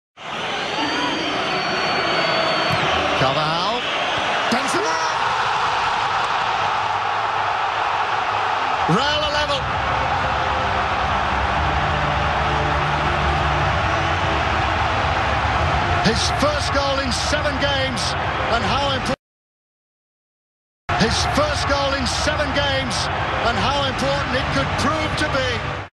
Karim Benzema Header Goal Real Madrid vs Napoli 1 1 UCL 15 02 2017 HD - New 1018
Real Madrid vs Napoli 1-1 Karim Benzema
เรอัลมาดริด VS นาโปลี 1-1 คาริมเบนเซม่า
مباراة ريال مدريد ونابولي 1-1 كريم بنزيمة
Реал Мадрид против Наполи 1-1 Карим Бензема